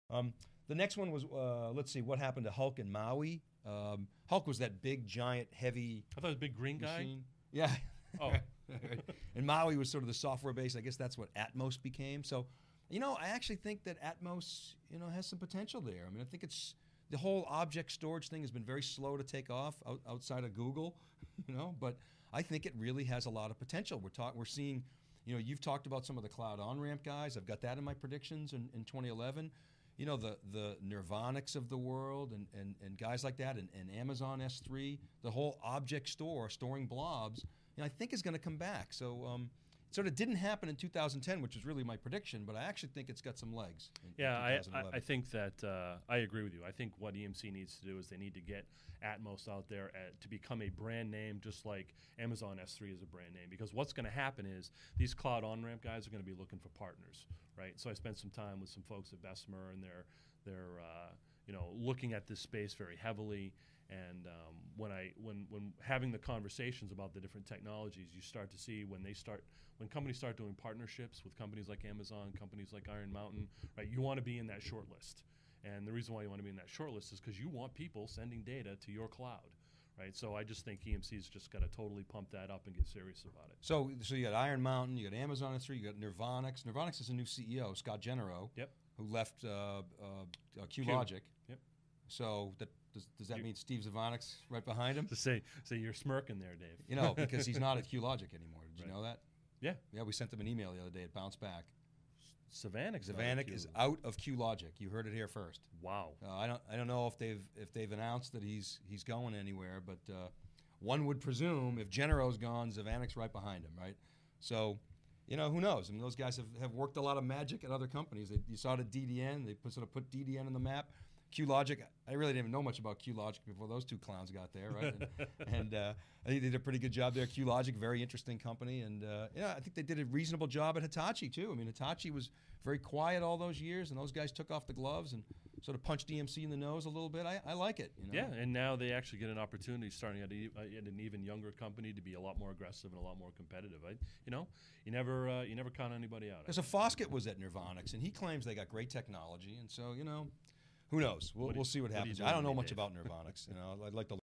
Is 2011 the Year of Cloud Storage?
Will cloud services from companies like Nirvanix, Amazon (S3) and Iron Mountain take off in 2011? Steve Kenniston, The Storage Alchemist and Wikibon's Dave Vellante talk about cloud storage predictions for 2011.